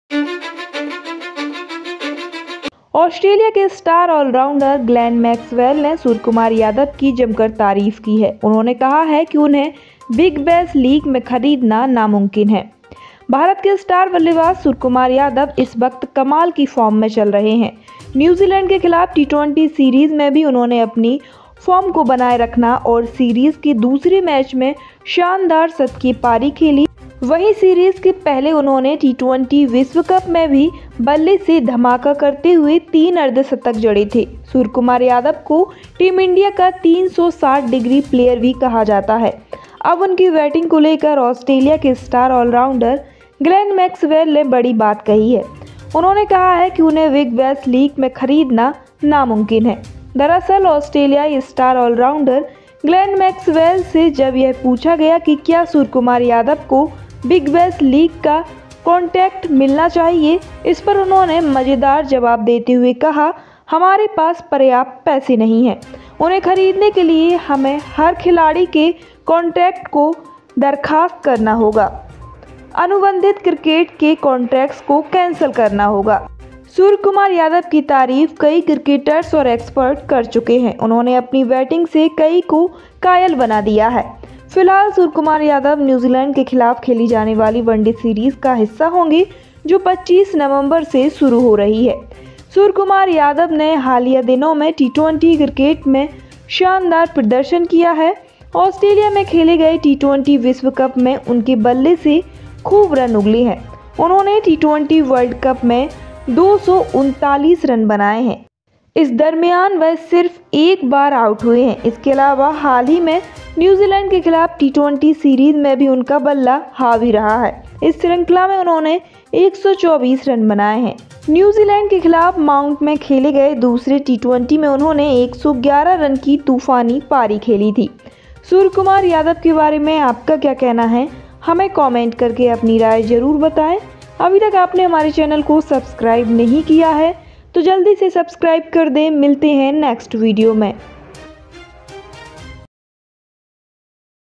Suryakumar yadav पर ग्लेन मैक्सवेल बोले ||ऑस्ट्रेलिया ने लगाई 200 करोड़ की बोली। DCNews
Suryakumar yadav पर Australia ने लगाई 200 करोड़ की बोली || DCNews

suryakumar yadav
suryakumar yadav batting
suryakumar yadav century
surya kumar yadav century
suryakumar yadav 100 vs new zealand
suryakumar yadav century vs newzealand